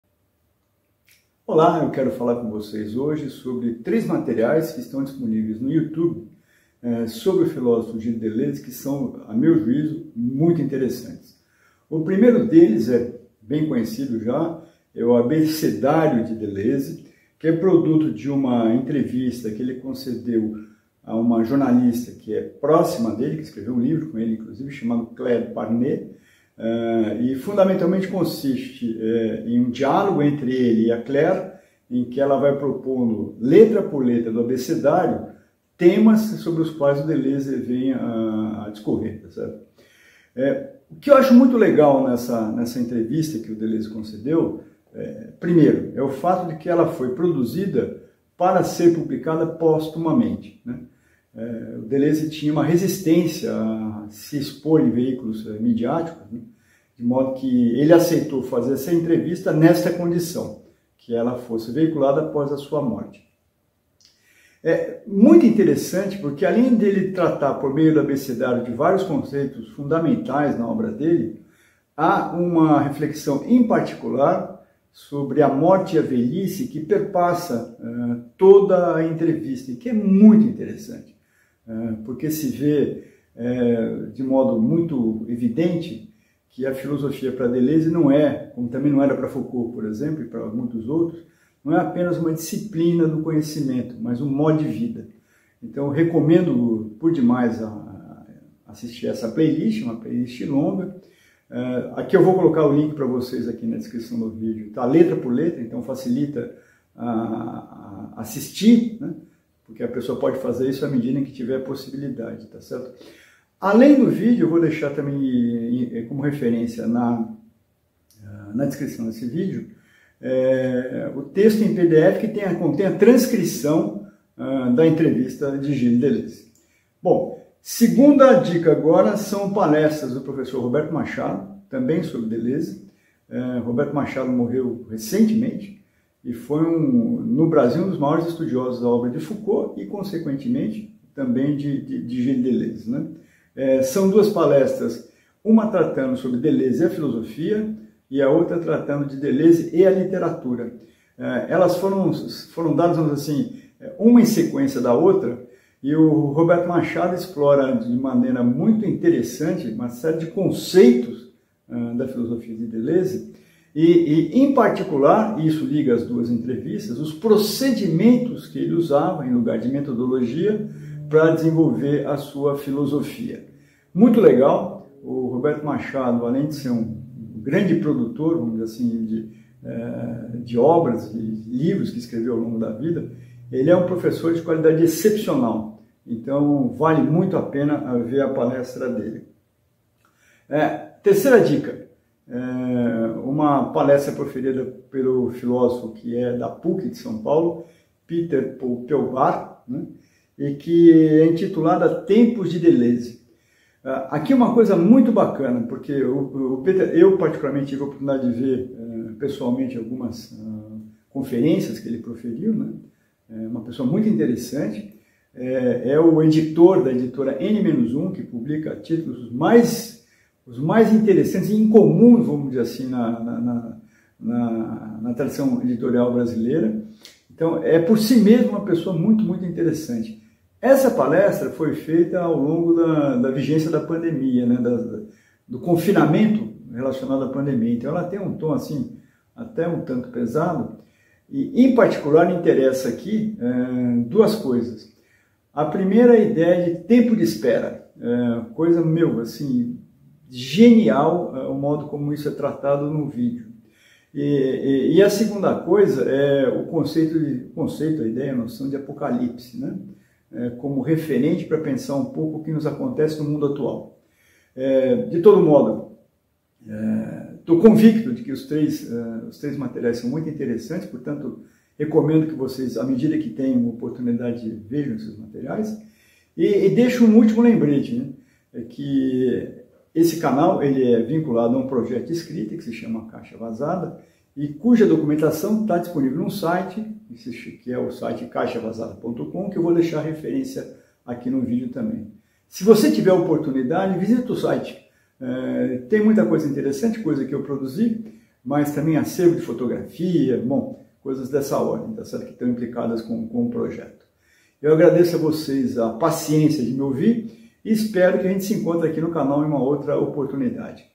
Dica de 3 vídeos sobre Deleuze
𝗢 𝗮𝗯𝗲𝗰𝗲𝗱𝗮́𝗿𝗶𝗼 𝗱𝗲 𝗚𝗶𝗹𝗹𝗲𝘀 𝗗𝗲𝗹𝗲𝘂𝘇𝗲
Entrevista concedida à jornalista Claire Parnet, para ser veiculada postumamente.

Referências
Transcrição em pdf

𝗣𝗮𝗹𝗲𝘀𝘁𝗿𝗮 𝗱𝗲 𝗣𝗲𝘁𝗲𝗿 𝗣𝗮𝘂𝗹 𝗣𝗲𝗹𝗯𝗮𝗿𝘁